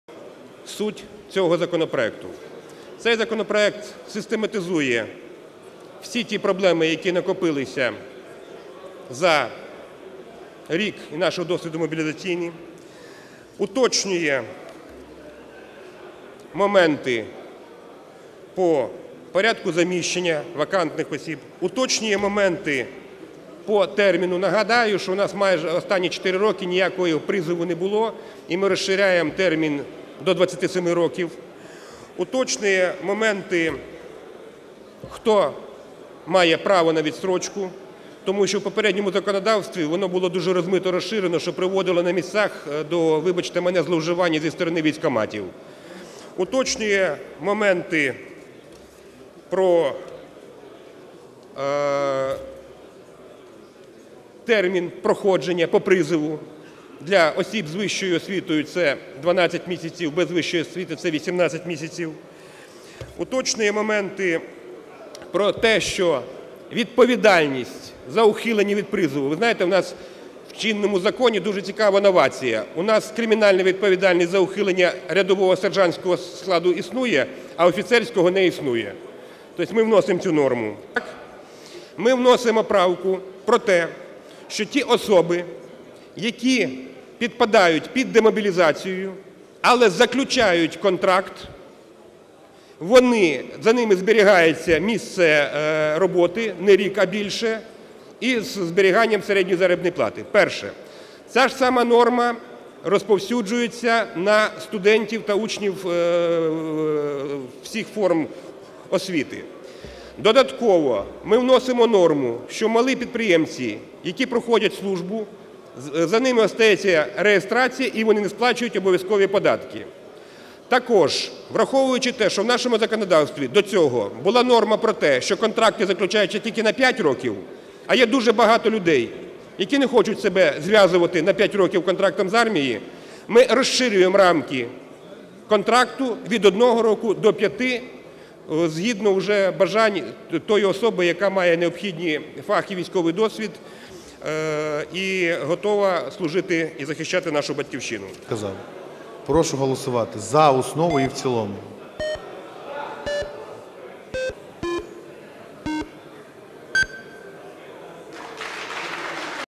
Зміни до закону про проходження військової служби підвищують обороноздатність держави
Приєднуйся!